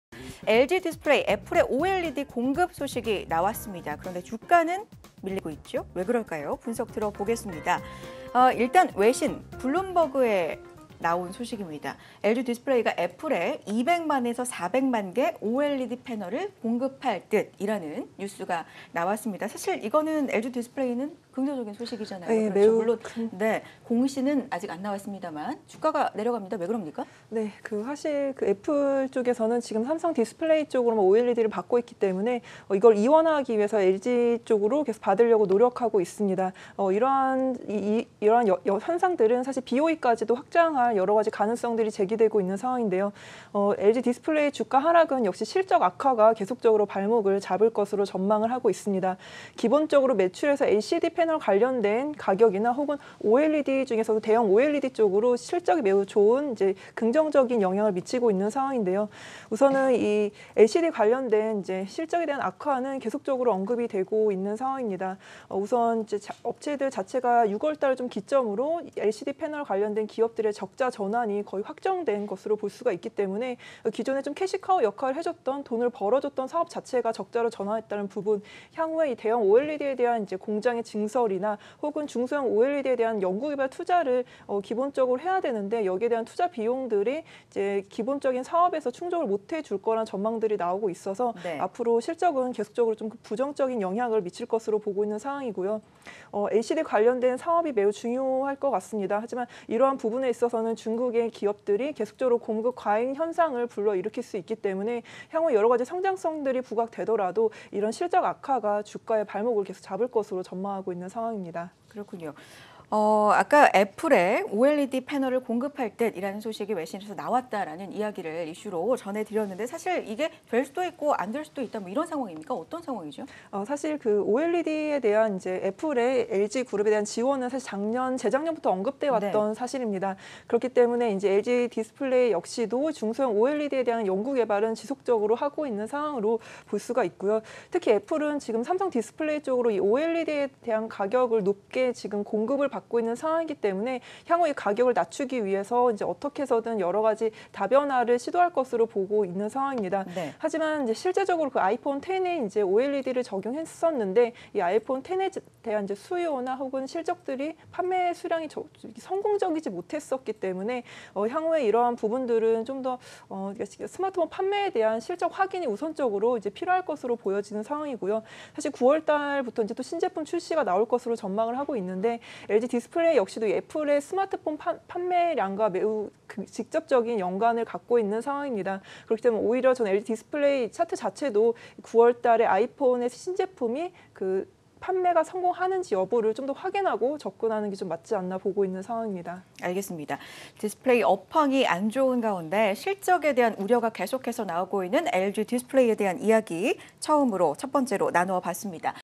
[이슈 될 이슈] LG디스플레이, 애플에 OLED공급? 그런데 주가는 왜?
[ 메르치종금증권 영업부 - 임정희 과장 ]